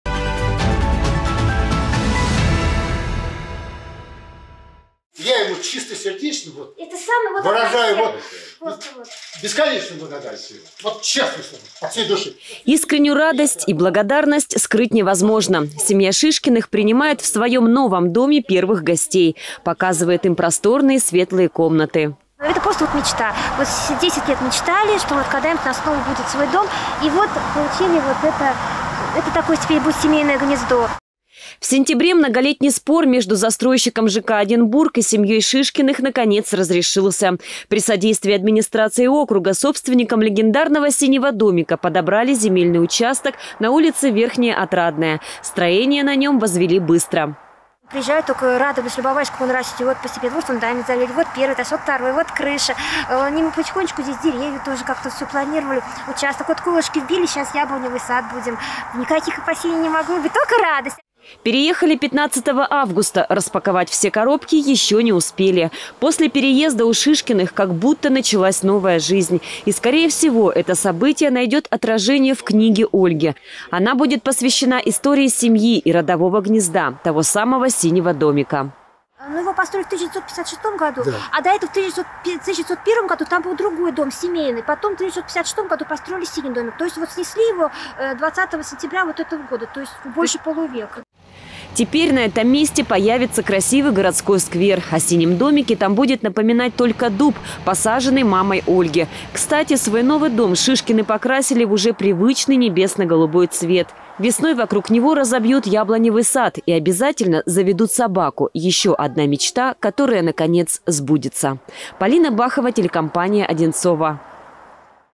В гостях у хозяев «Синего домика»
Глава округа Андрей Иванов побывал в гостях у семьи Шишкиных, хозяев «синего домика».